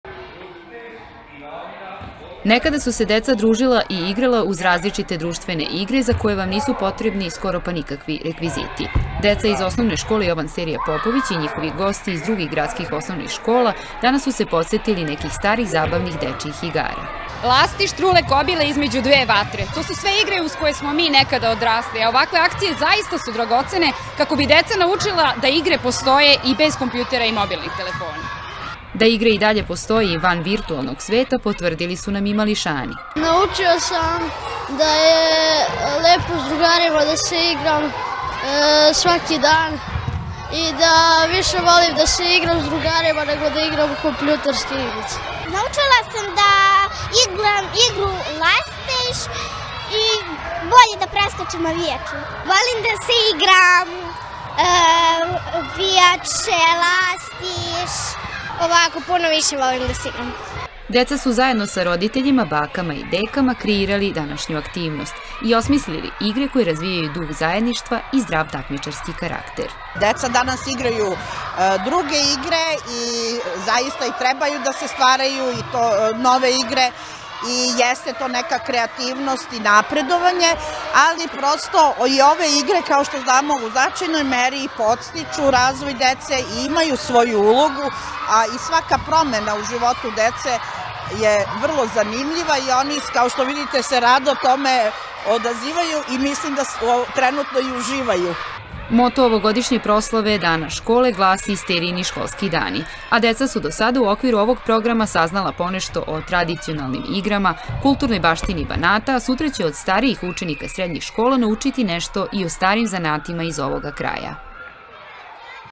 VRŠAČKI OSNOVCI ČUVAJU OD ZABORAVA STARE DEČIJE IGRE
OŠ “Jovan Sterija Popović” iz Vršca čitave ove nedelje obeležava Dan škole. Program je osmišljen tako da se deca  kroz različite aktivnosti upoznaju sa svim onim lepim tradicionalnim stvarima iz davno prošlih vremena, pa je tako danas organizovana aktivnost “Sačuvaj od zaborava”.